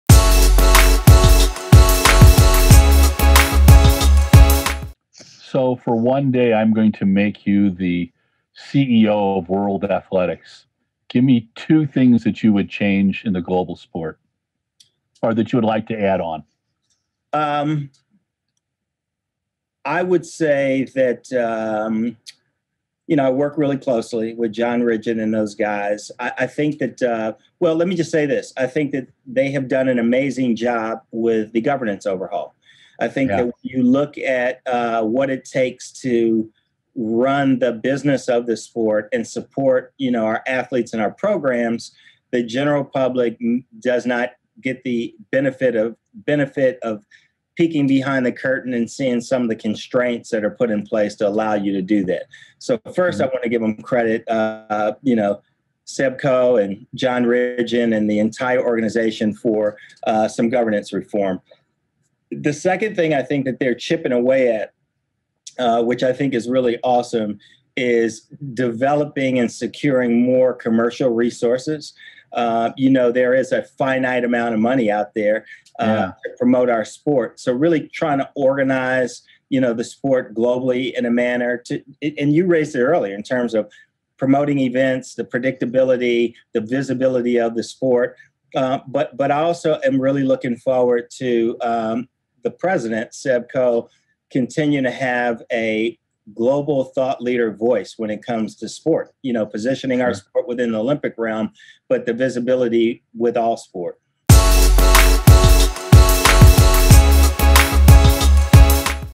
Socialing The Distance ft Max Siegel (CEO of World Athletics for a day)
RunBlogRun presents
Socialing The Distance with Larry Eder
Featuring Max Siegel, CEO of USA Track & Field
Clip 5 - CEO of World Athletics for a day

Find all of our Conversations with Larry SocialingTheDistance​ RunBlogRun​ theshoeaddicts​